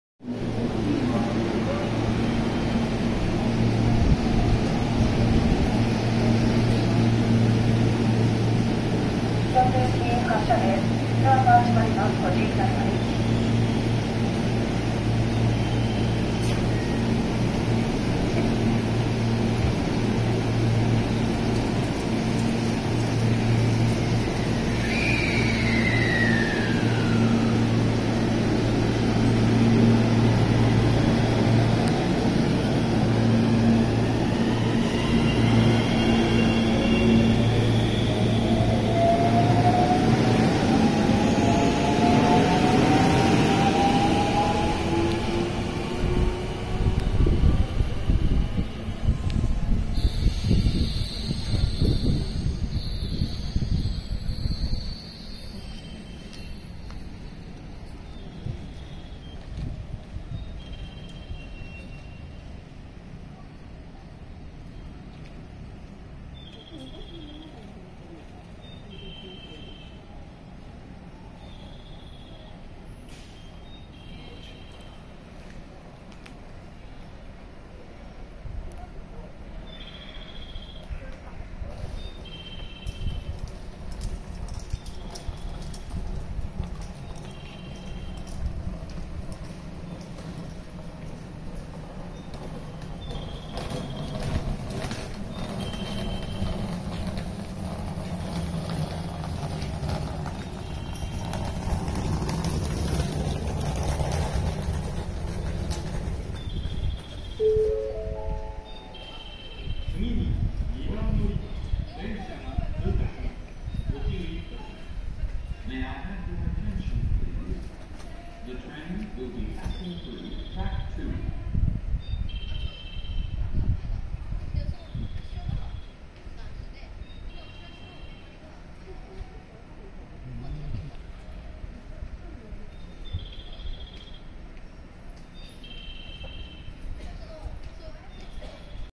JR西日本 223系6000番台 R203編成ダブルパンタ車 草津線からの普通 京都行き 膳所駅 後続普通列車20分遅延発生 20240924
JR西日本

223系6000番台 R203編成ダブルパンタ車
草津線からの普通 京都行き

膳所駅

後続普通列車20分遅延発生